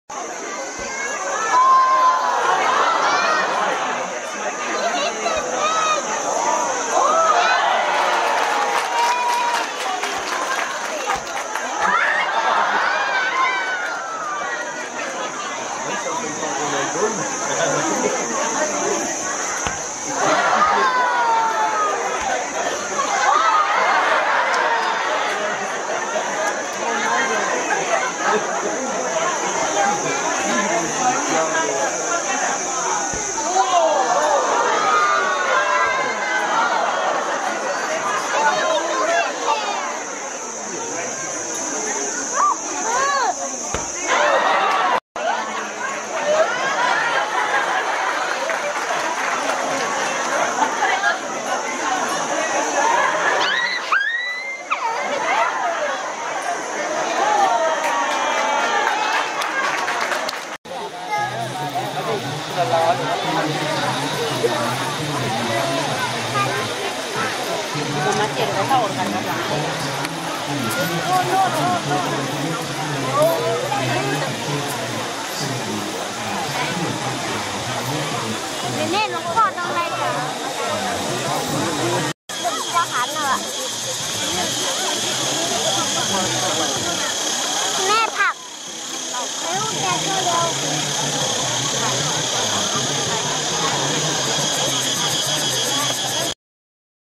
National Elephant Day in Mae Rim, Thailand
Thailand's National Elephant Day in Mae Rim, outside of Chiang Mai.